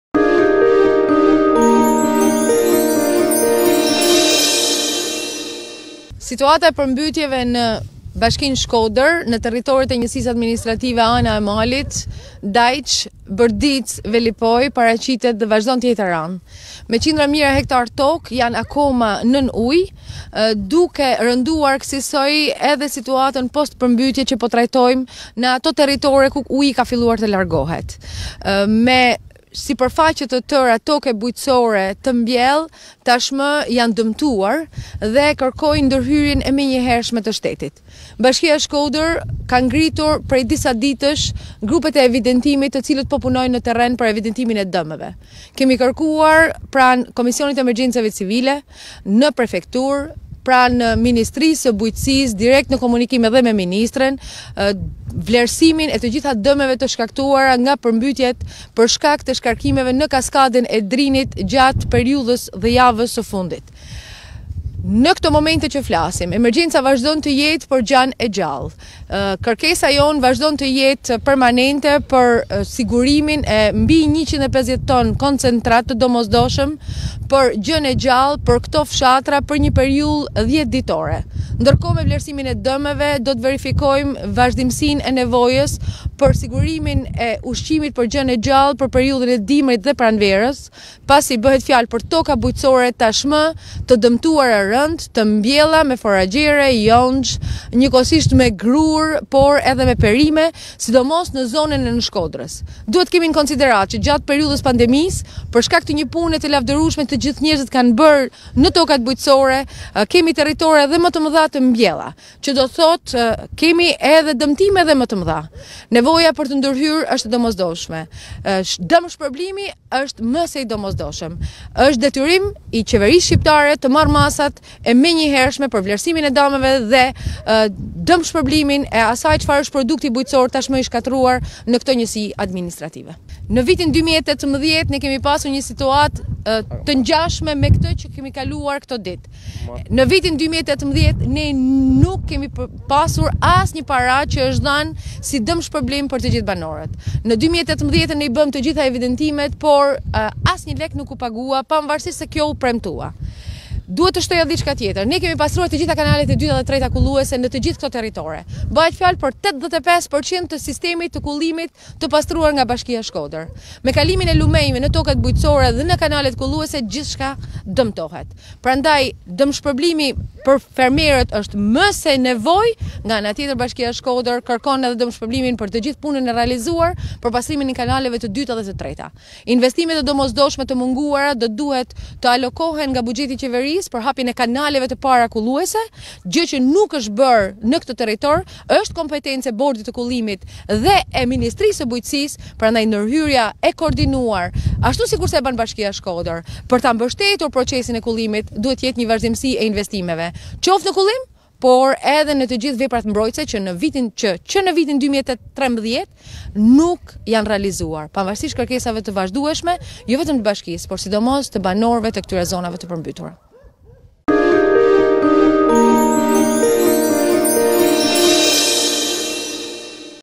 ShkodraWeb I Përmbytjet në Shkodër, Voltana Ademi kerkesë qeverisë: Dëmshpërbleni banorët e dëmtuar
Kryetarja e Bashkisë së Shkodrës Voltana Ademi ka qenë sot në zonën e Urrelave të Shirqit në Njësinë Administrative të Dajçit. Pasi ka takuar dhe bashkëbiseduar me banorët e fermerët, ajo ka dalë me një kerkesë specifike në emër të tyre.

Në një lidhje direkte, ajo ka kërkuar nga qeveria dëmshpërblim nga qeveria për banorët që kanë pësuar dëme. Po ashtu, Ademi ka kujtuar kërkesën e saj për mbi 150 ton koncentrat për gjënë e gjallë për të paktën 10 ditë. Ajo ka shtuar se duhet menduar edhe për muajt e dimrit e pranverës.

Kryebashkiakja ka kujtuar se janë pastruar 85% e kanaleve të dyta dhe të treta kulluese nga qeverisja vendore por uji i lumit në kanale dhe toka, ka dëmtuar këtë rrjet dhe për këtë, ka kërkuar dëmshpërblim nga qeveria.